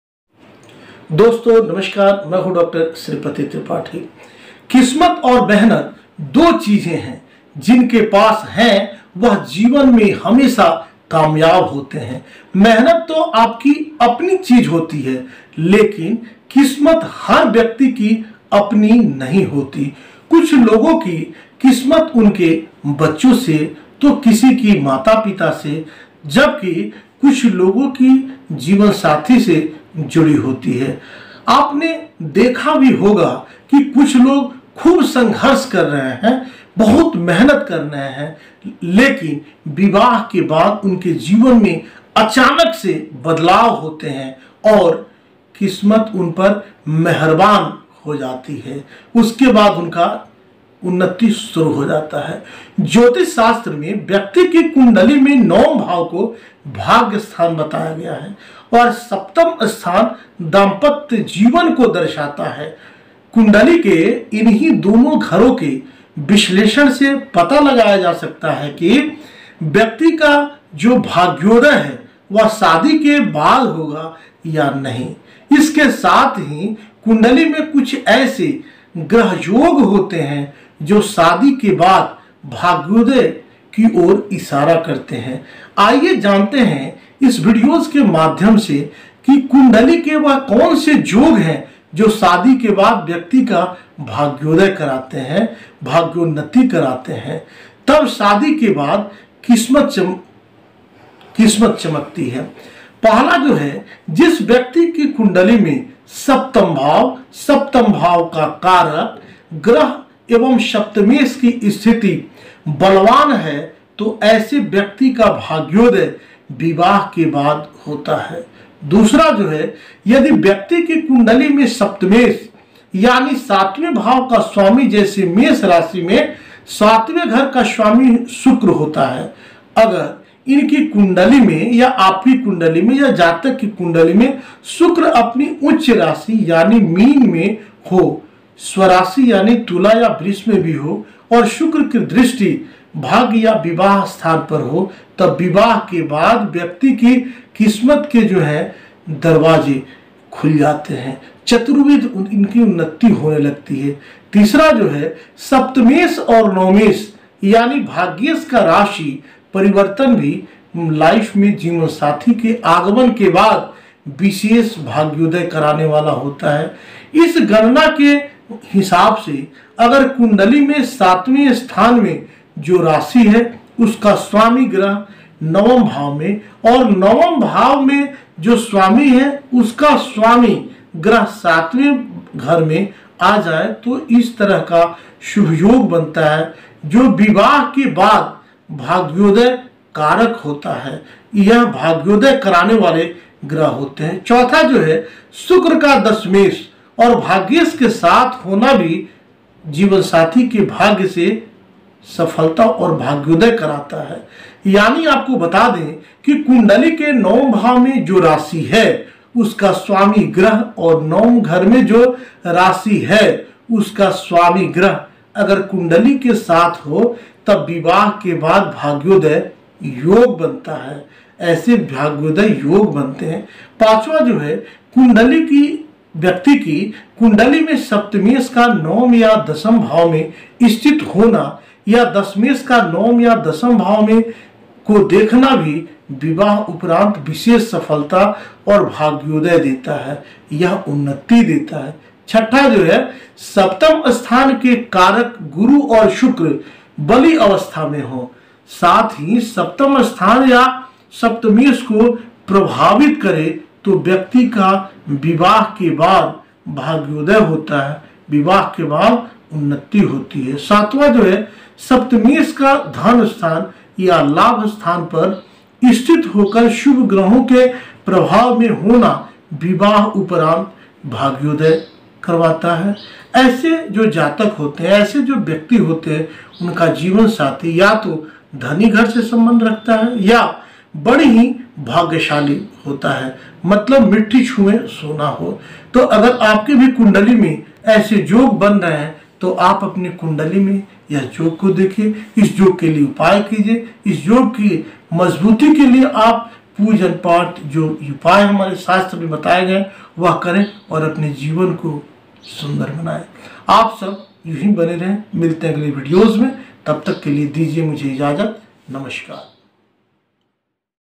कुंडली के ये योग शादी के बाद कर देता है मालामाल | Kundali ke ye yog shadi ke baad banta hai malamal
दोस्तों, आज इस वीडियो के माध्यम से हम आपको बताने वाले है कि कुंडली के ये योग शादी के बाद कर देता है मालामाल. आपको बता दे कि आप अक्सर देखें होंगे की बहुतों का जीवन शादी के बाद अचानक बदल जाता है. क्या आपने कभी सोचा है आखिर ऐसा क्यों होता है.